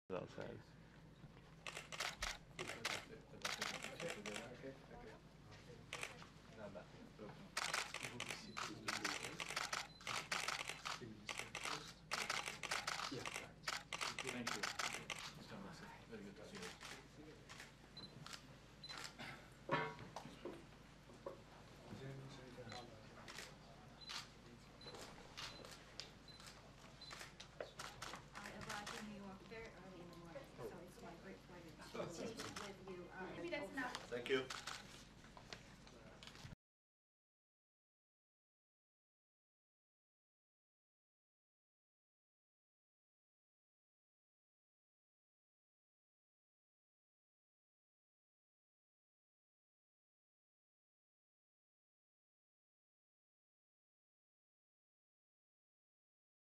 Chinese Foreign Minister Meets UN General Assembly President
Chinese foreign Minister Wang Yi met with Miroslav Lajcak, president of the 72nd session of the UN General Assembly, in New York City on Monday.